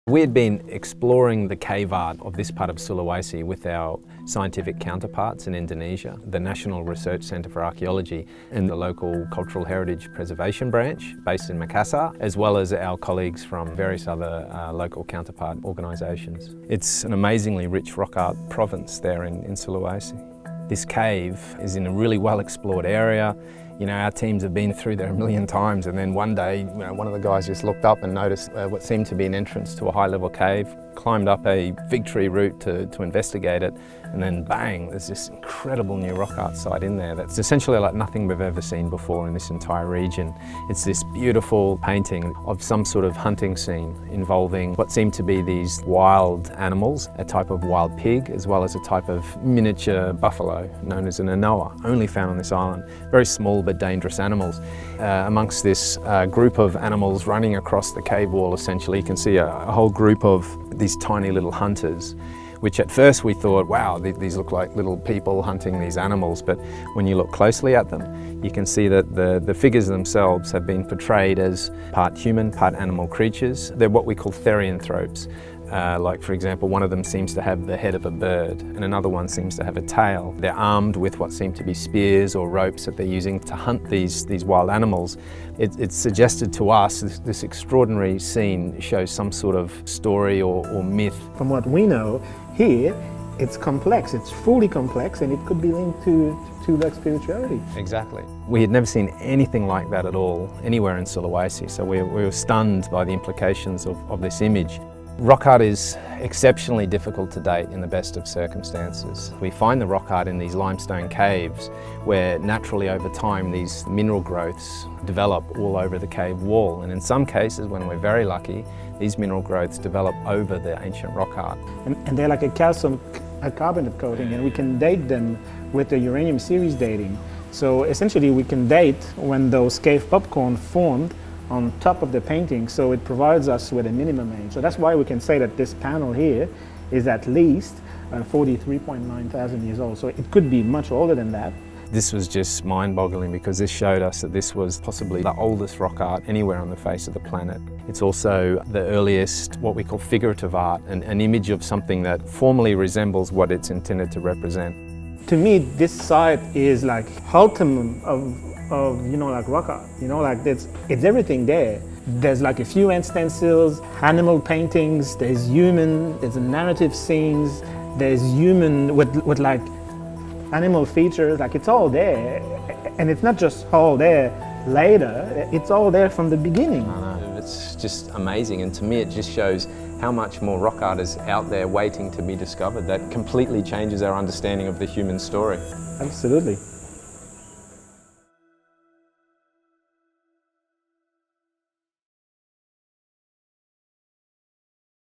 Earliest hunting scene in prehistoric art
A team of archaeologists led by Griffith University has discovered a cave painting in Indonesia that is at least 44,000 years old and which portrays a group of part-human, part-animal figures – ‘therianthropes’ – hunting large mammals with spears or ropes, casting new light on the origin of modern human cognition.